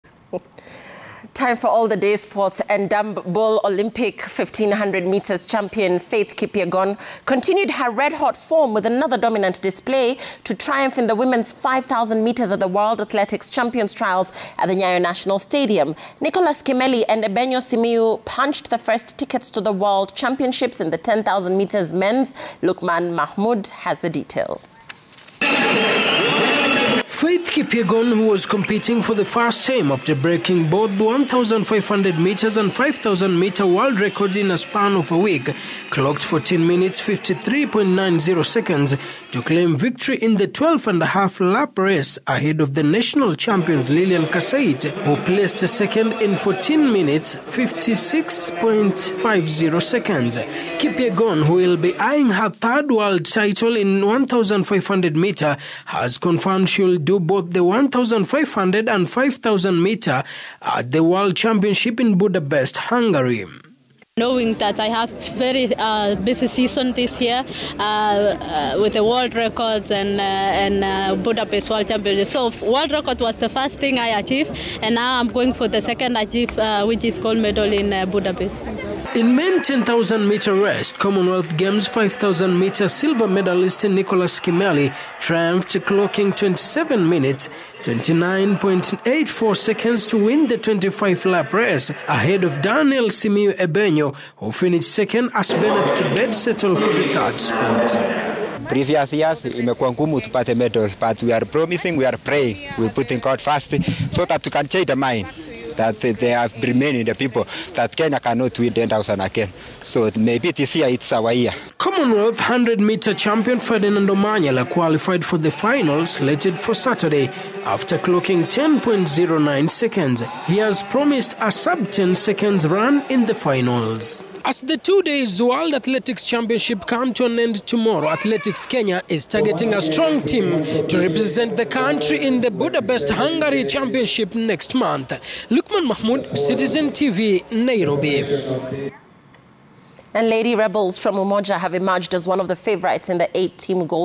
Faith Kipyegon makes it in 5000m women
Nicholas Kimeli and Daniel Ebenyo qualify in 10000m